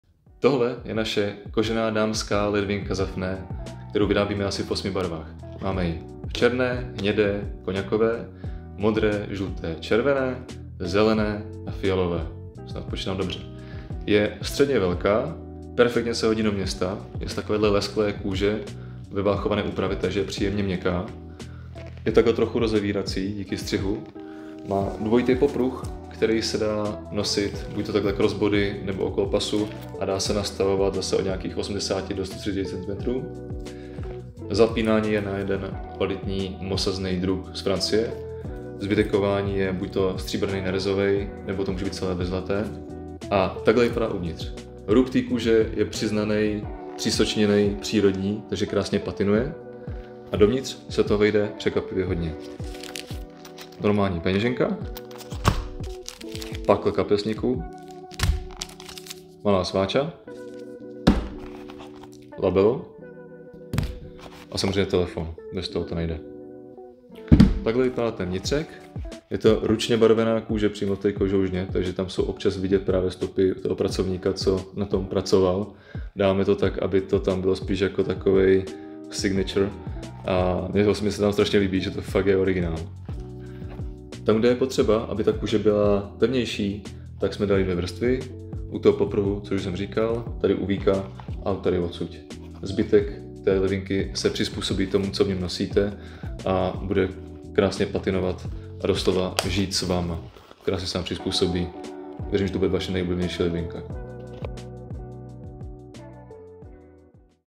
Kožedělník - Dámská ledvinka Zafné
Minimalistická a na své rozměry prostornější ledvinka z měkčí hovězí kůže v krásně živých barvách.